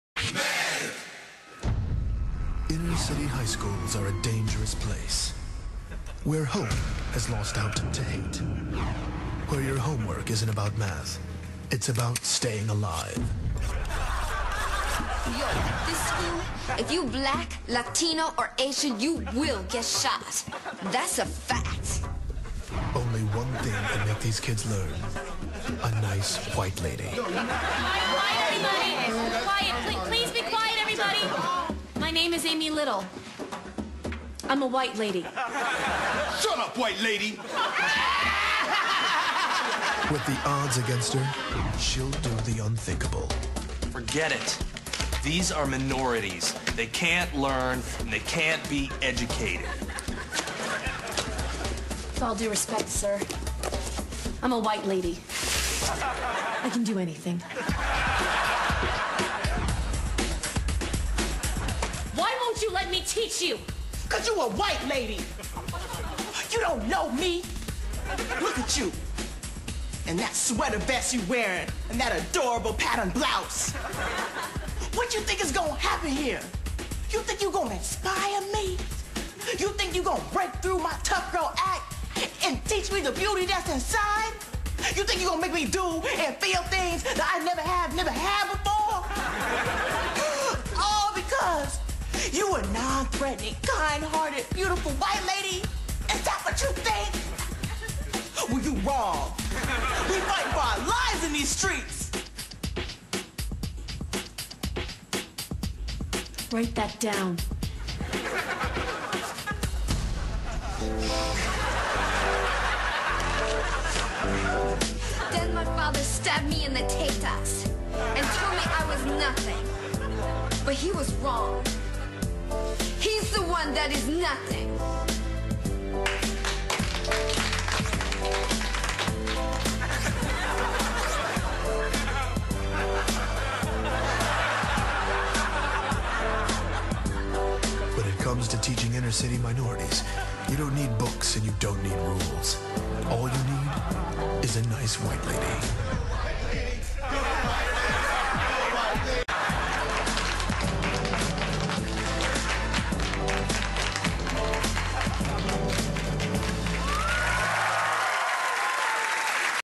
Nice White Lady
Parody on a bunch of feel-good teacher movies, but mainly Freedom Writers stands out.  Hilarious.